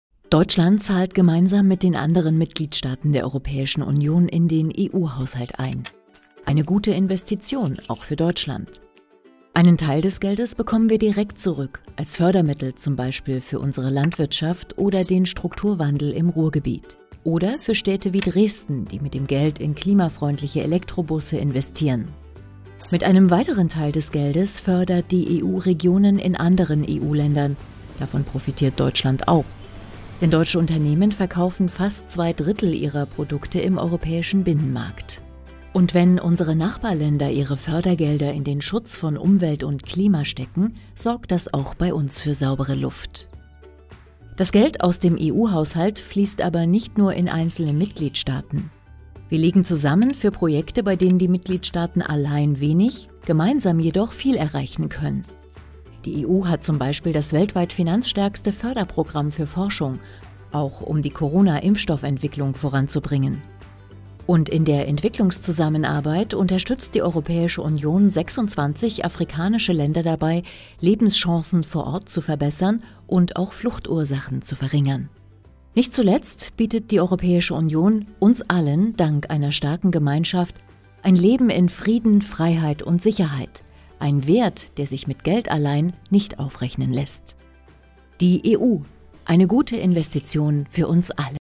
In 100 Sekunden: Die EU - eine gute Investition für uns alle
Die starke Gemeinschaft der Europäischen Union ermöglicht ein Leben in Frieden, Freiheit, Sicherheit und mit Fortschritt. Dafür hat sich Deutschland im vergangenen halben Jahr während der EU-Ratspräsidentschaft tatkräftig engagiert. Warum Investitionen in die EU eine gute Investition für uns alle sind - Europa verstehen in 100 Sekunden.